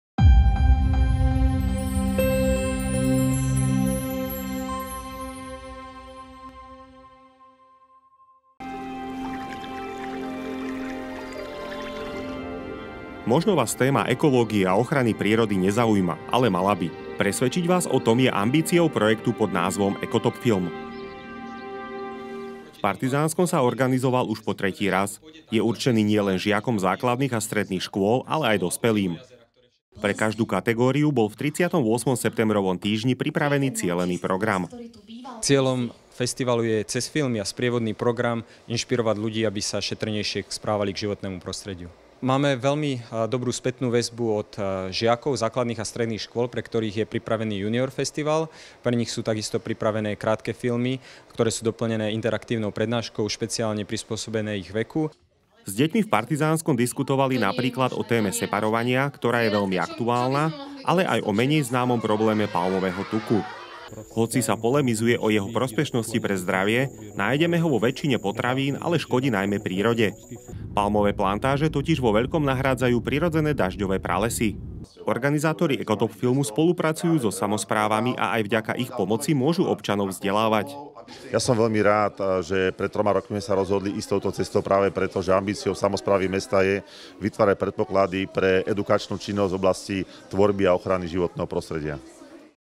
Nielen o probléme palmového tuku
S deťmi v Partizánskom diskutovali napríklad o téme separovania, ktorá je veľmi aktuálna, ale aj o menej známom probléme palmového tuku.